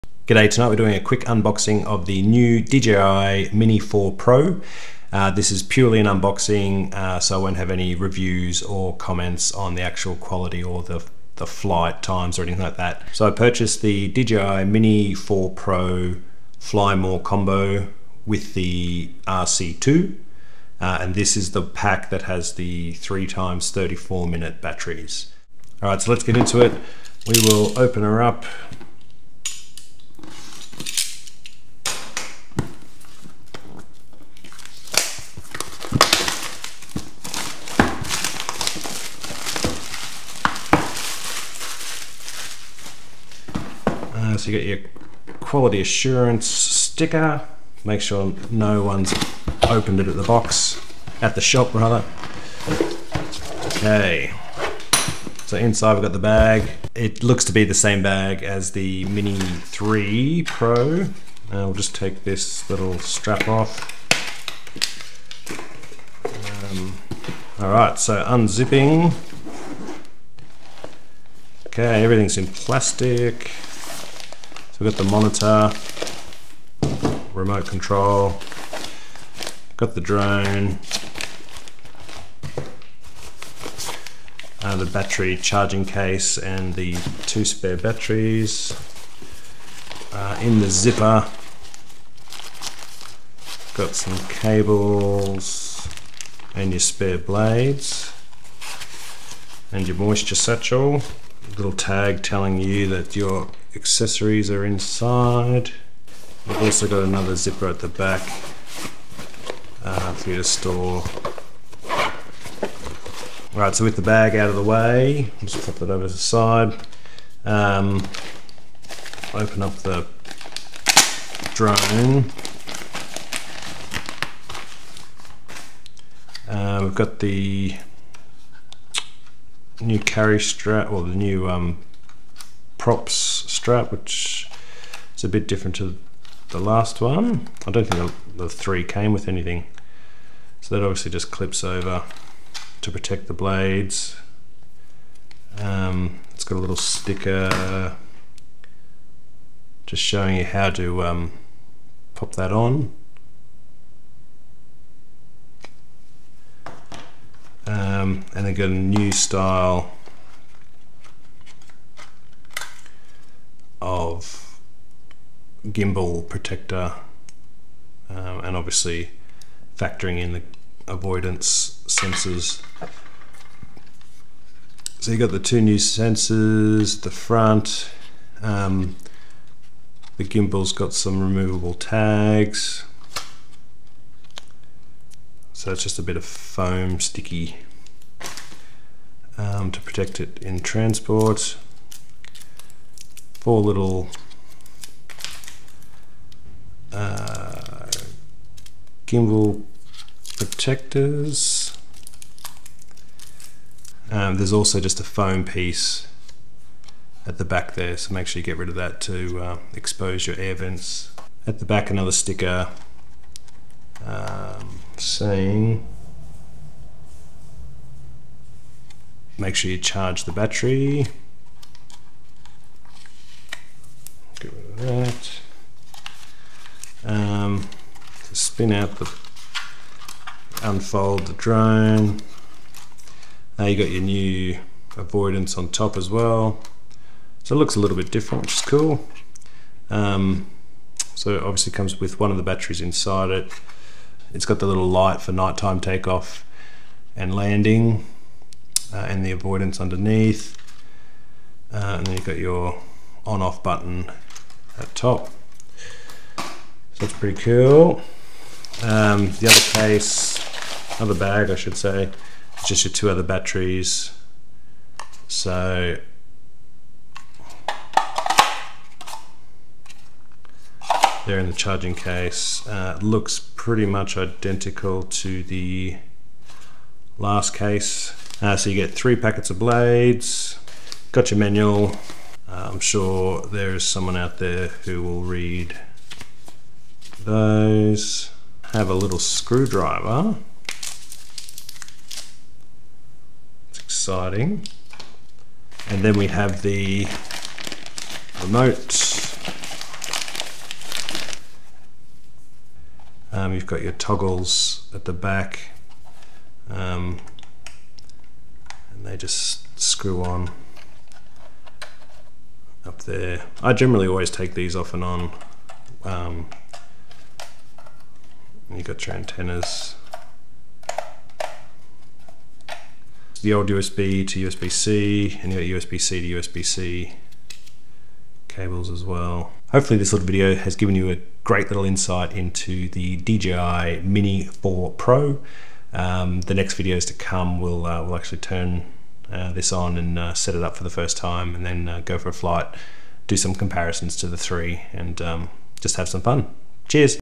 DJI Mini 4 Pro - UNBOXING
A simple video unboxing the DJI Mini 4 Pro.
See what's inside the box and become as excited to see the screwdriver as I was.
It's has a couple of differences to the DJI Mini 3 Pro as well.

00:00 - 00:25 Intro
00:25 - 00:56 Opening the box
00:56 - 1:11 Unzipping the bag
1:11 - 1:53 Pulling drone, RC and batteries out
1:53 - 4:18 Looking over the DJI mini 4 pro
4:18 - 4:38 Looking at new DJI mini 4 pro batteries
4:38 - 4:55 Accessories and screwdriver
4:55 - 5:23 DJI RC 2
5:23 - 5:29 DJI USB C cables
5:29 - 5:47 Outro